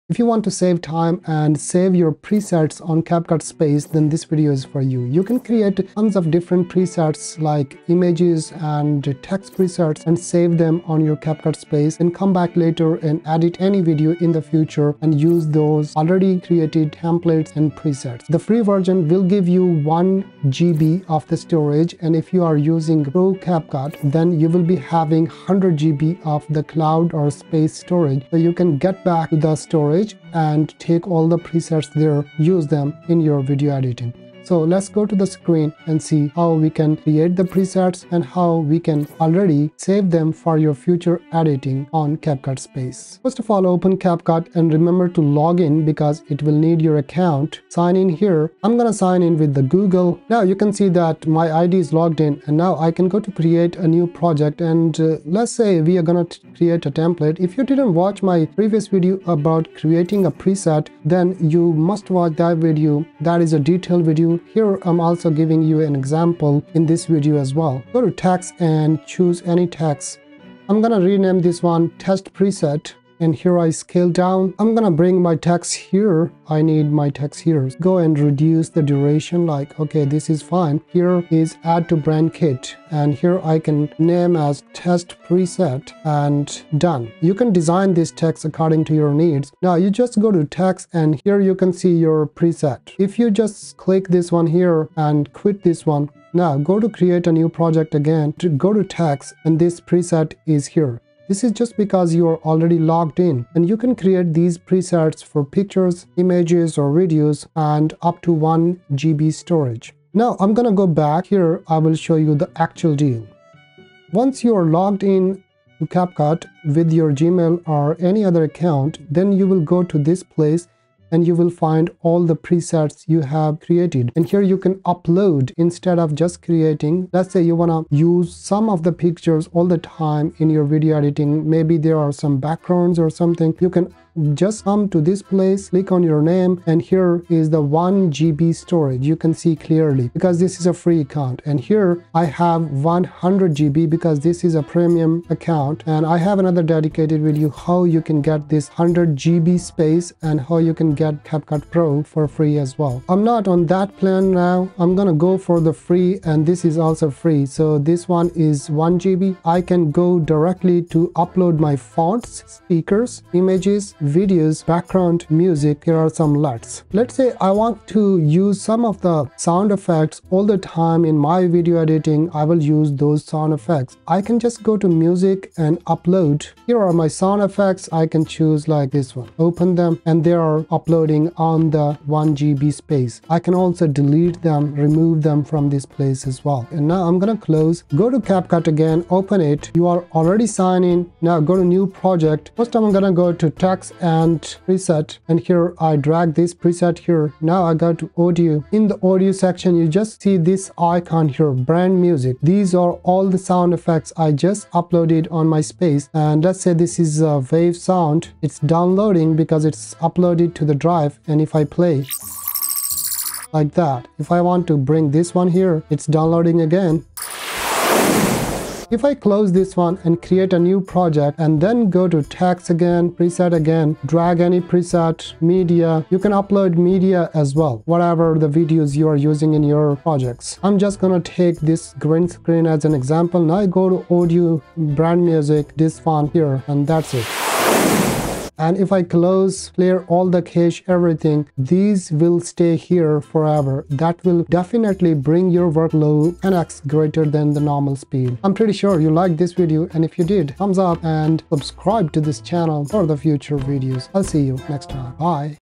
How To Create Presets in CapCut For Faster Video Editing (Brand Kit Tutorial)
Unlock the Secrets of CapCut Brand Kit: Your Ultimate Guide to Reusable Presets & Lightning-Fast Editing via CapCut Cloud.

Are you tired of spending hours creating and recreating the same edits for your videos? Do you wish there was a way to streamline your editing process and save precious time? Well, look no further than CapCut Brand Kit, your key to reusable presets and effortless video editing.

In this comprehensive video, you'll embark on a journey to mastering CapCut Brand Kit, discovering the power of saving presets, LUTs, images, and music to your CapCut Cloud storage. With just a free account and 1GB of storage, you'll be equipped to organize your creative assets and streamline your editing workflow like never before.

Here's what you'll learn in this game-changing video:
1. Unleash the Power of Presets: Learn how to create and save a variety of presets, including text presets, to effortlessly apply consistent styles across your videos.
2. Optimize Your Editing Workflow: Discover how to utilize your saved presets to edit videos with unmatched speed and efficiency, saving you valuable time and effort.
3. Harness the Power of CapCut Cloud: Explore the vast capabilities of CapCut Cloud, your one-stop shop for storing and managing all your creative assets.
4. Embrace Effortless Editing: Experience the freedom of editing with ease and precision, thanks to the power of reusable presets and organized assets.
Whether you're a seasoned video editor or just starting out, this video is your essential guide to unlocking the full potential of CapCut Brand Kit. Say goodbye to time-consuming editing processes and embrace the future of lightning-fast video creation.
Links to the videos promised in this video:
Don't miss out on this opportunity to revolutionize your editing workflow and transform your video creation journey. Watch now and unleash your editing mastery!